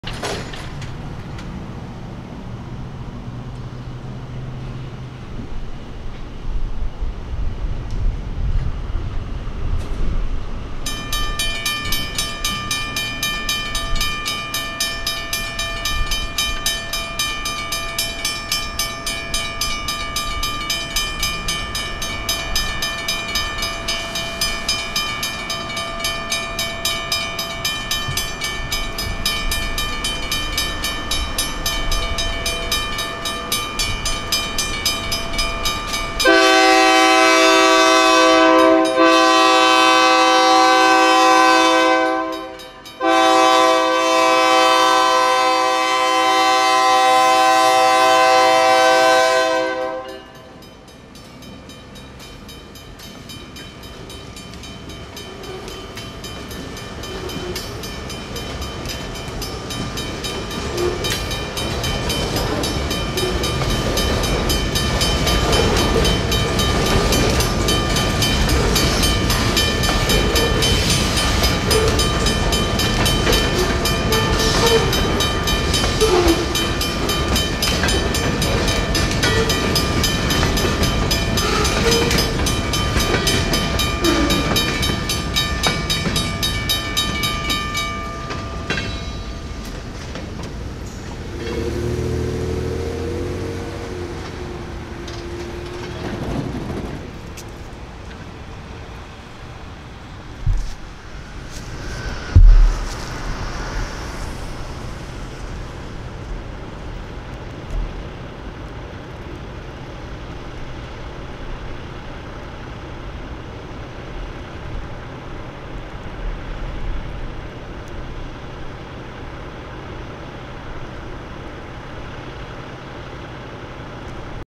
CFNR 2401 Davis Job Local, Santa Anita Dr. Railroad Crossing, Woodland CA - Nice Sounding WRRS Bells
[Filmed 12:42pm June 28, 2021]
After doing some long time switching on a siding off an industry lead track, the train shoved back onto this spur track past this crossing to do some switching and I caught them coming back out passing the crossing here. This crossing is an SPRR 1960s to 1970s signal build install with changes to its gates only.

California Northern Railroad Davis Job Local freight train passes through the Santa Anita Drive railroad crossing on a CFNR spur track in Woodland, California.

California Northern Railroad
CFNR 2401 SE24B Tier-4/5

Crossing Info
City/State: Woodland, California
Location: Santa Anita Dr & Churchill Downs Ave
Horn Setting: Horn Zone
Crossing Owner and Maintainer: California Northern Railroad
Bell Sequence: Ring till gates start rising.
Track Activation Sensors: Island Circuit Sensors
Train Line: CFNR Lead Spur

Right Signal
Bell: WRRS Mechanical Bell
Cantilever: Harmon (1960s to 1970s)
Lights: Four pairs of 8x20 inch Harmon Incandescent Lights
Brackets: Harmon
Junction Boxes: Harmon
Gated Mast
Gate Lights: WCH Incandescent Lights
Counterweight Arms: WABCO
Mechanism: WABCO Modal 75
Base: Safetran

Left Signal
Brackets: Harmon
Junction Boxes: Harmon
Gated Mast
Counterweight Arms: WABCO
Base: Safetran

Camera
Sony Cybershot RX10 IV - Takstar SGC-598 Mic